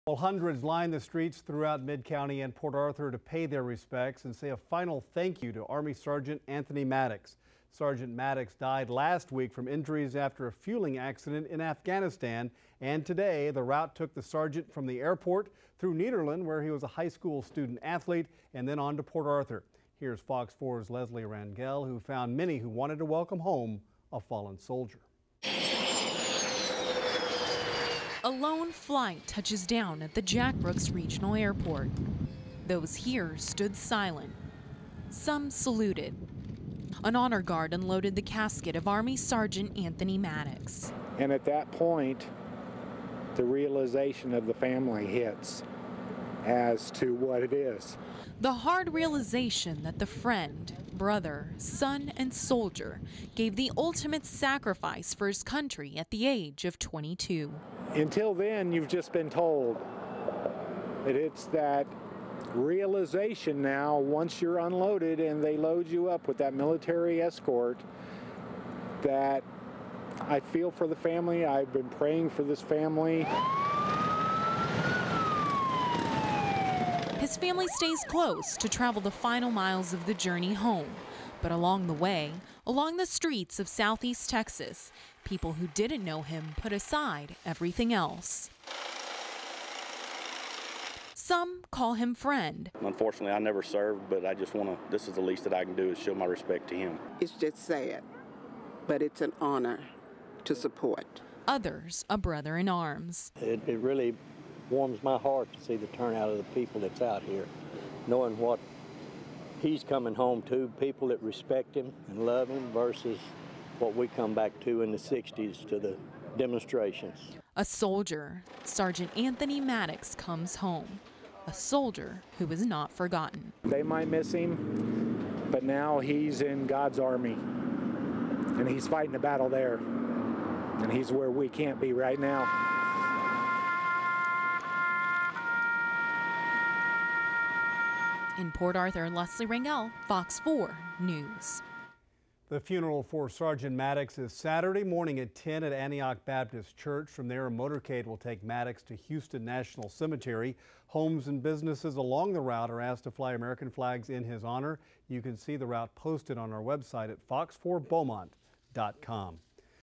Fallen soldier Sgt. Anthony Maddox returns home
The body of a fallen soldier returned to Southeast Texas this morning.

Sergeant Anthony Maddox died July 22nd following a fuel explosion in Afghanistan.

Southeast Texans paid tribute to the soldier this morning, Leslie Rangel was there along the honorary route.